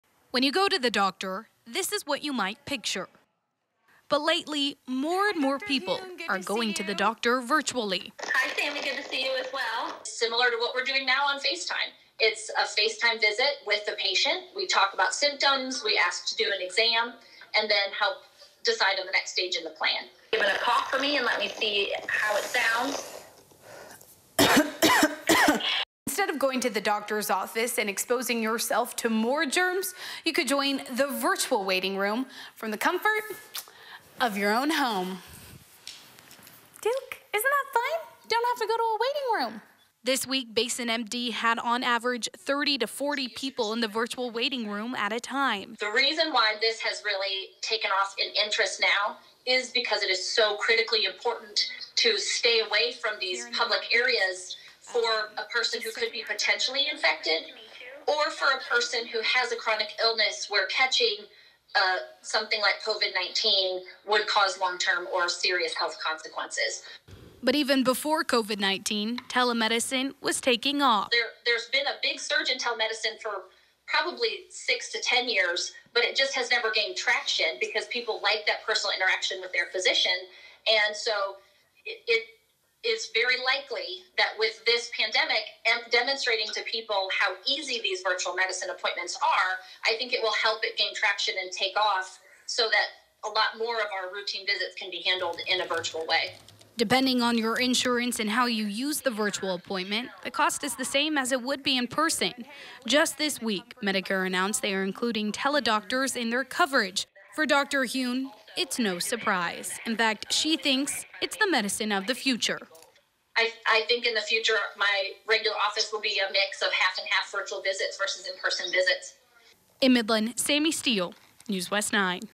Tele-doctors becoming more common in West Texas due to COVID-19
Instead of waiting in a waiting room exposed to excess germs, patients can join the virtual waiting room from home the comfort of their own home.